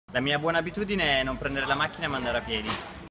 lefaccedellebuoneabitudini. Simone fotografo dj
I consigli dei testimonial di contagiatidibenessere la seconda campagna social di @crpiemonte e Statigeneralidellosport per un corretto stile di vita.